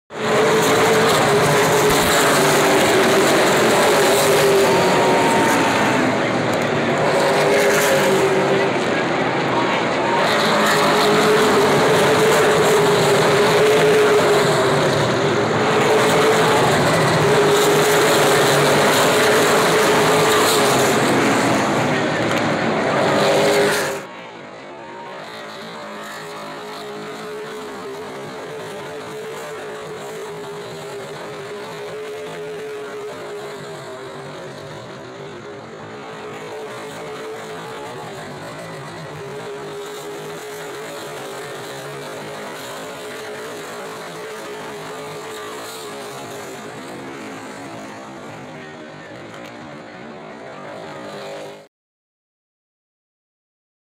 COLE BUTCHER AND MIKE HOPKINS AT HICKORY
Mike Hopkins and Cole Butcher fight for the win in the closing laps of the Pro All Stars Series St. Patrick's Day 150 at Hickory Motor Speedway.